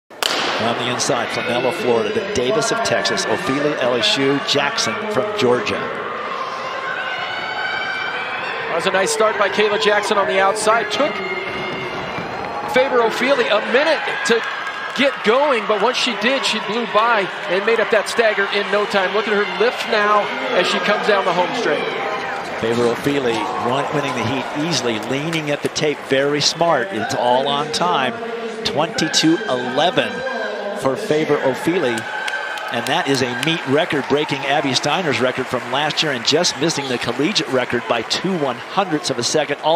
Favour Ofili - 22.11 - 200 Meters
Favour Ofili runs a PR of 22.11 at the 2023 NCAA Championships in Albuquerque, N.M.